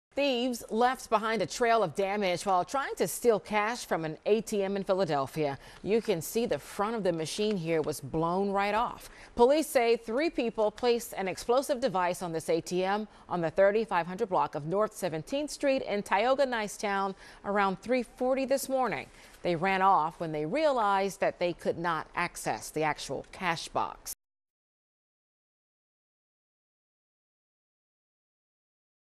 Thieves use explosives in failed attempt to steal cash from ATM
Thieves left behind a trail of damage while trying to steal cash from an ATM in Philadelphia.